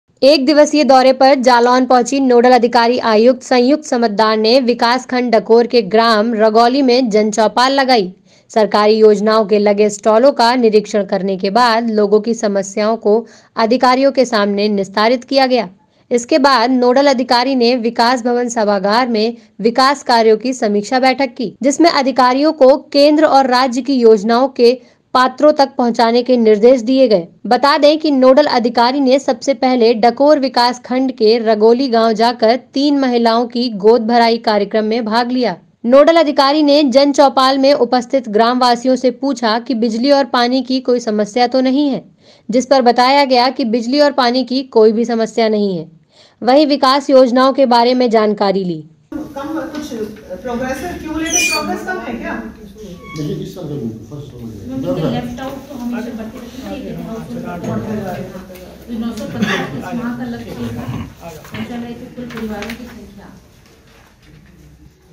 Jalaun : नोडल अधिकारी का दौरा रगौली गांव में लगाई जनचौपाल ||abnews india
एक दिवसीय दौरे पर जालौन पहुँची नोडल अधिकारी आयुक्त संयुक्त समद्दार ने विकास खंड डकोर के ग्राम रगौली में जनचौपाल लगाई... सरकारी योजनाओं के लगे स्टालो का निरीक्षण करने के बाद लोगों की समस्याओं को अधिकारियों के सामने निस्तारित किया गया.